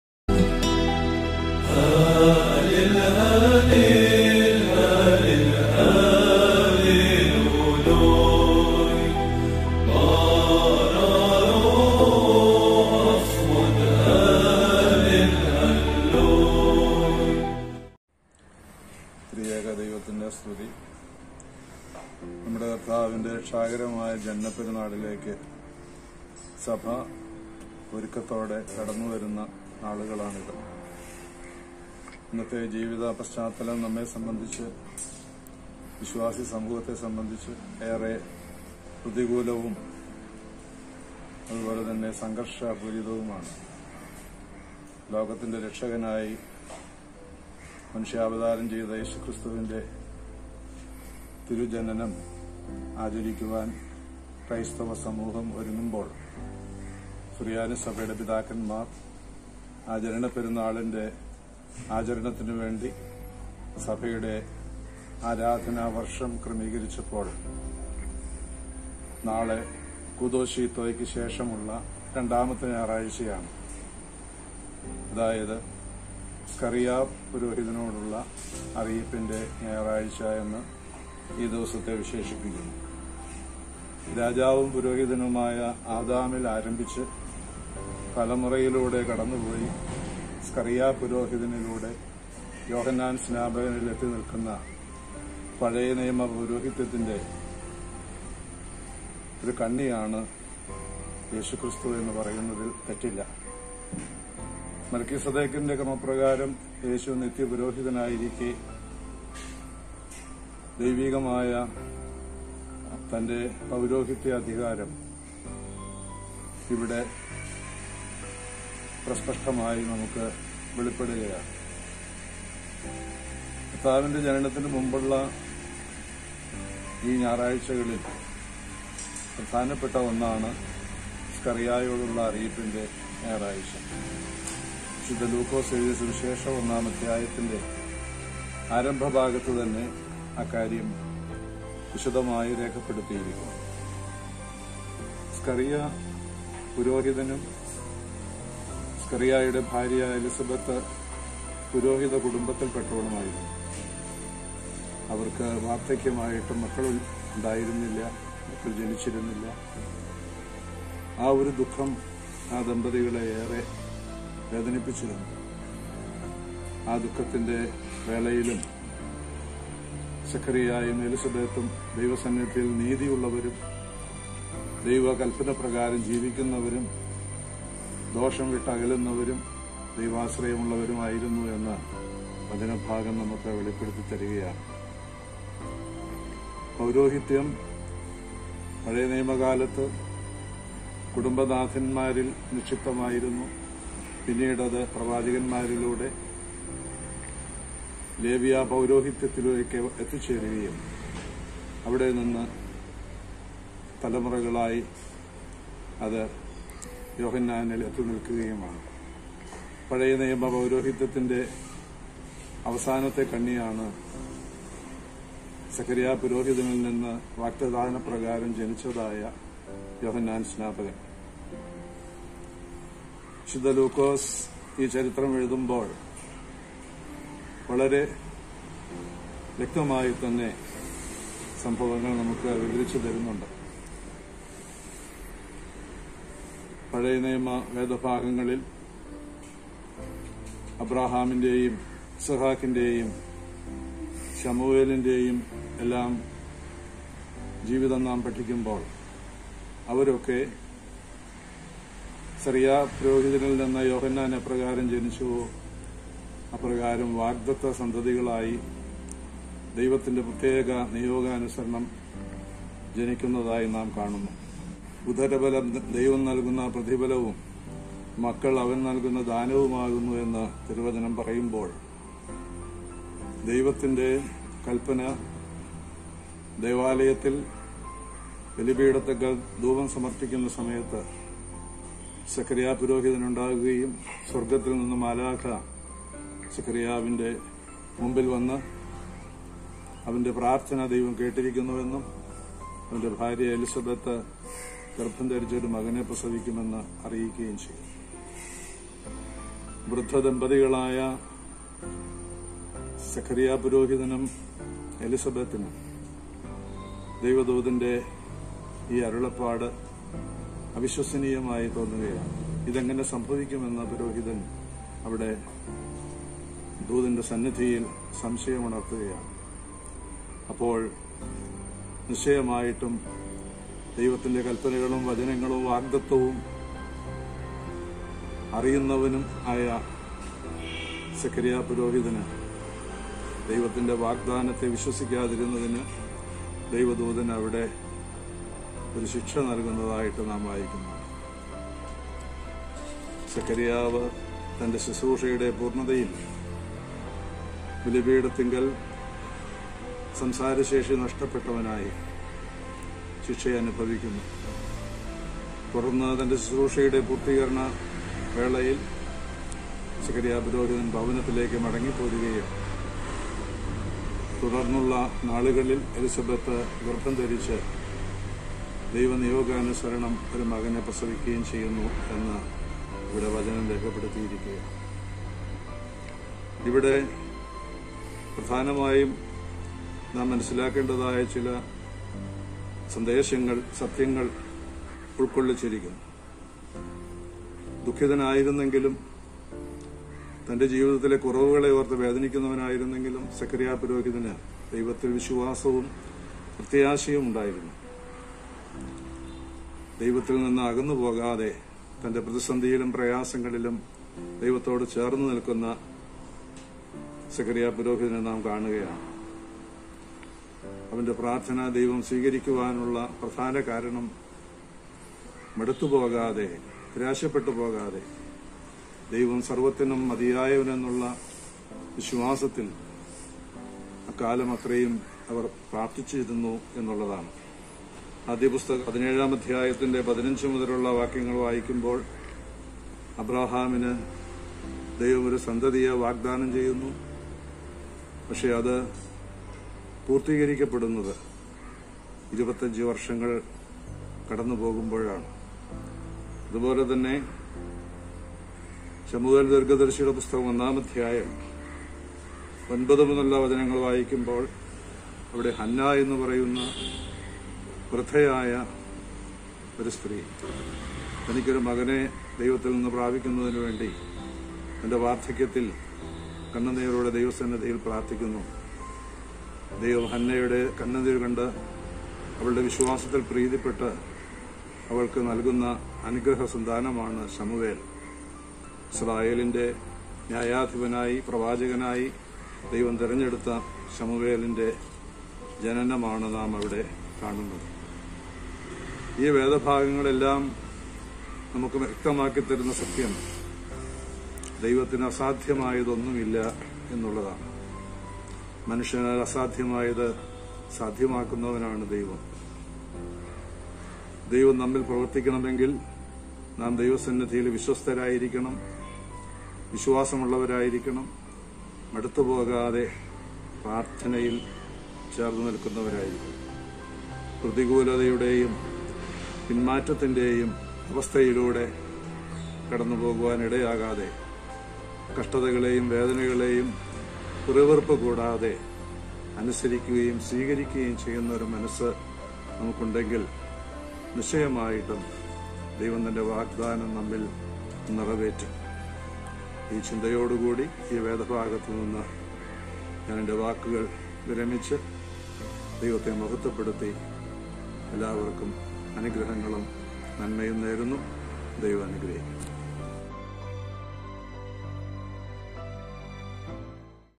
പ്രതിവാര ചിന്ത | റവ. ഫാ. വർഗ്ഗീസ്‌ പുലയത്ത്
സ്കറിയയോടുള്ള അറിയിപ്പിന്റെ  ഞായർ  | വി.ലൂക്കോസ് 11:45-51